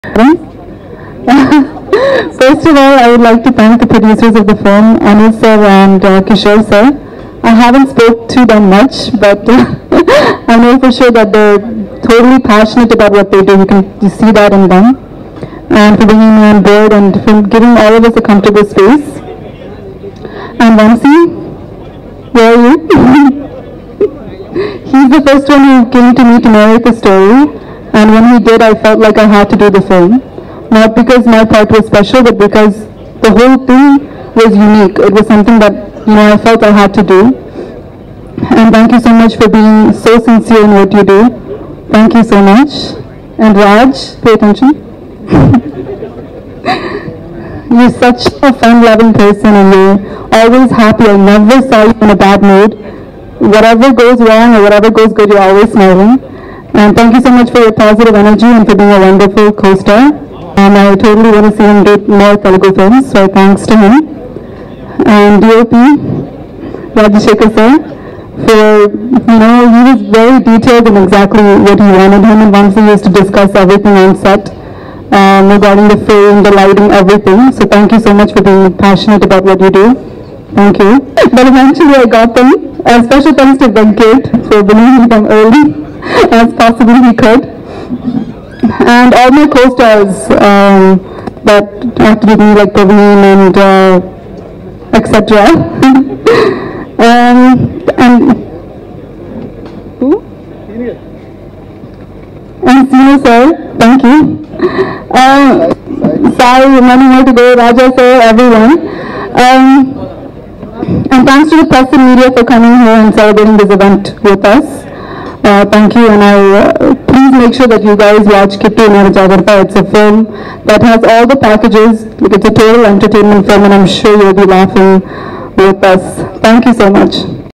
Anu Emmanuel Lovely Speech @ Kittu Unnadu Jagratha Movie Gummadikaya Function
Kittu Unnadu Jagratha Movie Gummadikaya Function
Starring: Raj Tarun & Anu Emmanuel
Directed by Vamsi Krishna.
Produced Under The Banner A.K.Entertainments.